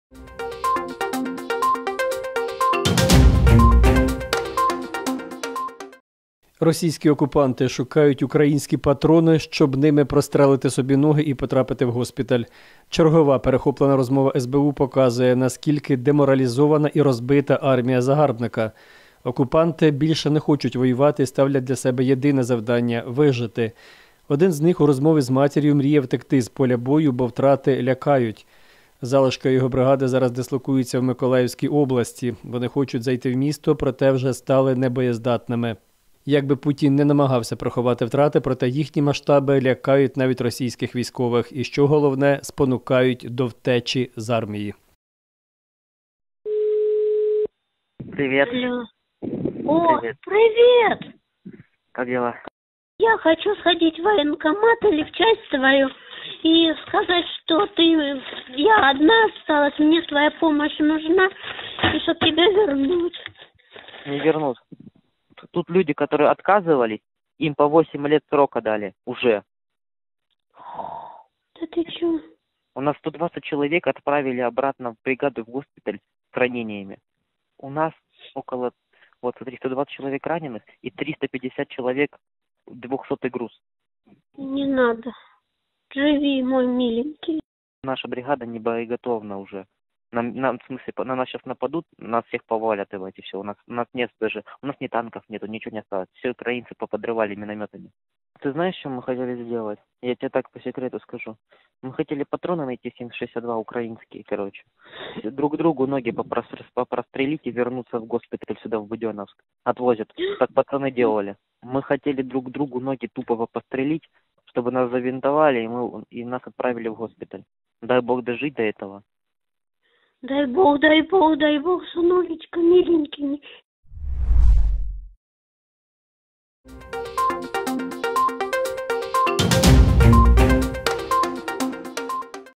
Російські окупанти шукають «українські» патрони, щоб прострелити собі ноги і потрапити в госпіталь
Російські окупанти шукають «українські патрони», щоб ними прострелити собі ноги і потрапити в госпіталь.

Чергова перехоплена розмова СБУ показує, наскільки деморалізована і розбита армія загарбника. Окупанти більше не хочуть воювати і ставлять для себе єдине завдання – вижити.

Один з них у розмові з матірю мріє втекти з поля бою, бо втрати лякають. Залишки його бригади зараз дислокуються в Миколаївській області. Вони хочуть зайти в місто, проте вже стали небоєздатними. Як би путін не намагався приховати втрати, проте їхні масштаби лякають навіть російських військових. І що головне – спонукають до втечі з армії.